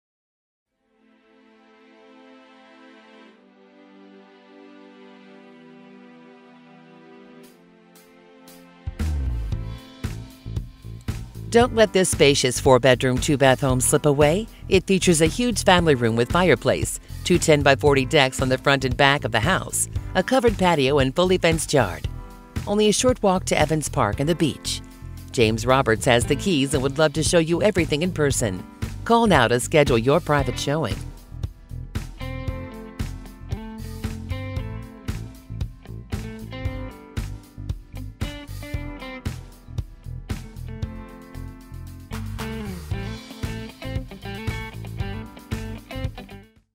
9122 Iroquois Evans, NY | MLS# B1128611 | MetroRoberts.com
9122 Iroquois, Evans, NY

Don't let this spacious home slip away!  Four large bedrooms, two full baths, huge family room with fireplace, two 10X40 decks on the front and back of the house, covered patio, fully fenced yard.  Walk only minutes to the Evans park and beach.  Enjoy Connors Hot Dogs, Turtle Joe's, and Mickey Rats.